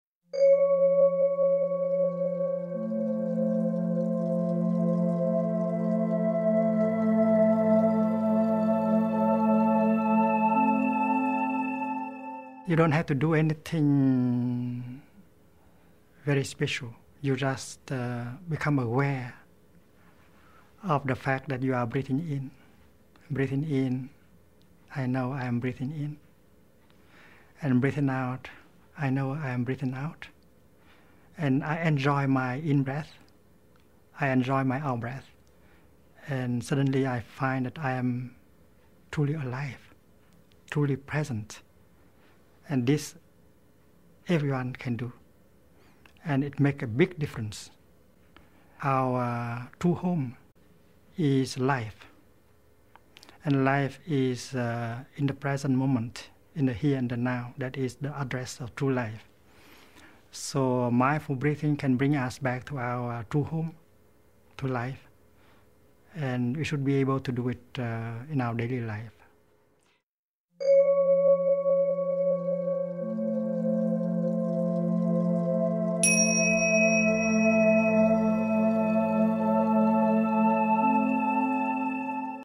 Thich Nhat Hanh - The Daily Path Podcast from MyPath TV
Meet with celebrated peace activist and Buddhist monk, Thich Nhat Hanh.